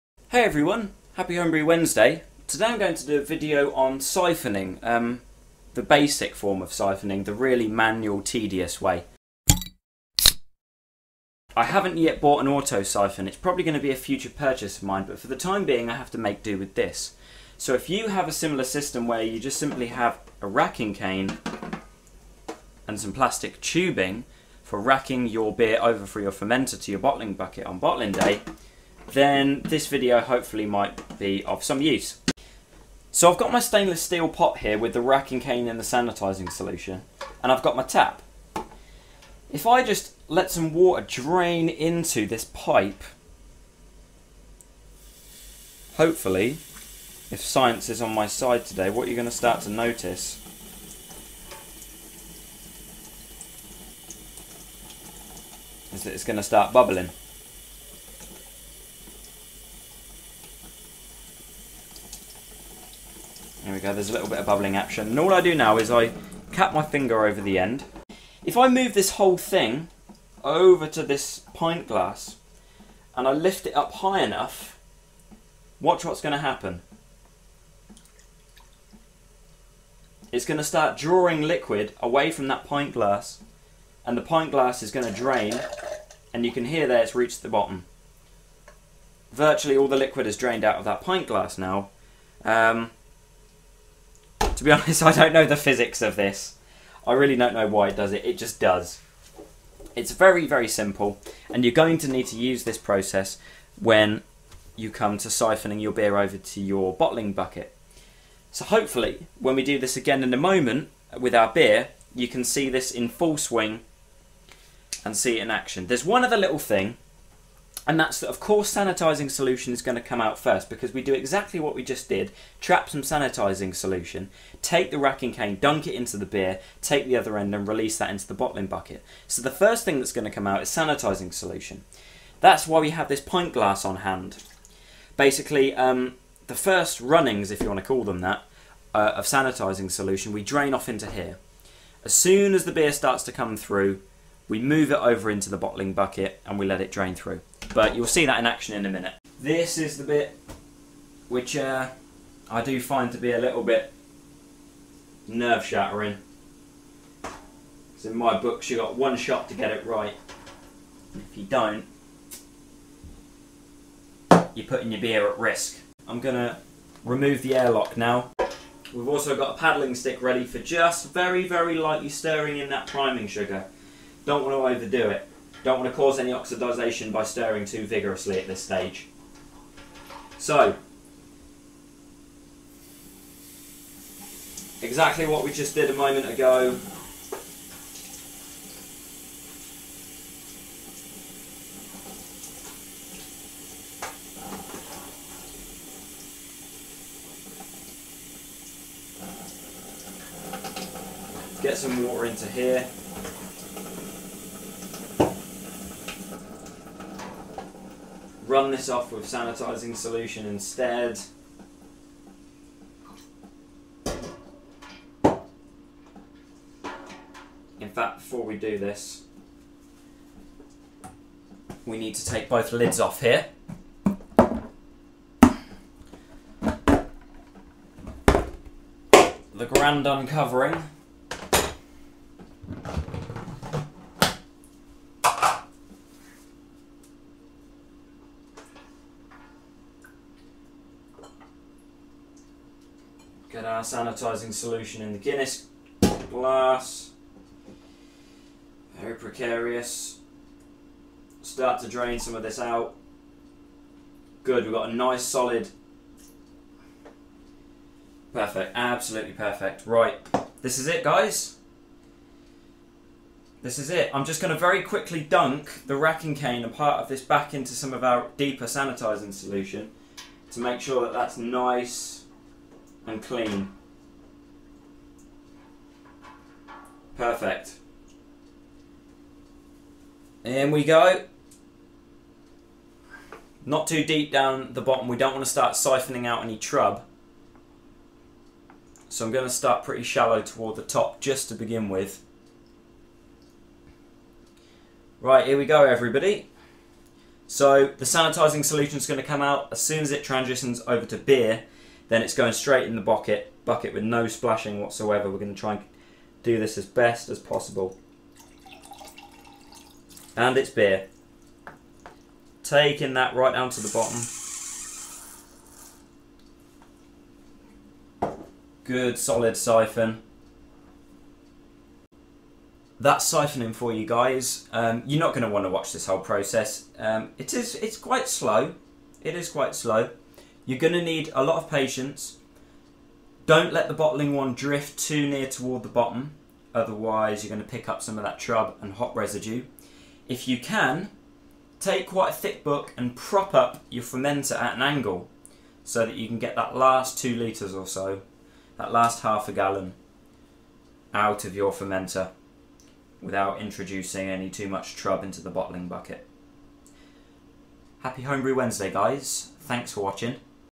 HBW #10 - Racking beer the manual way
Useful for racking to secondary fermentation too.
I'm fully aware auto-syphons are cheap and FAR less hassle, but right now its another £10 I can't afford!!!